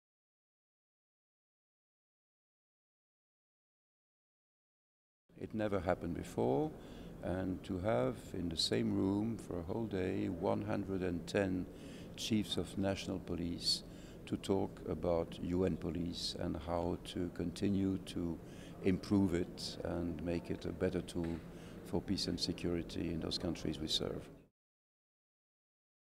Hervé Ladsous - Under-Secretary-General for Peacekeeping Operations
Mr. Hervé Ladsous, Under-Secretary-General for Peacekeeping Operations speaks about the significance of the first-ever United Nations Chiefs of Police Summit.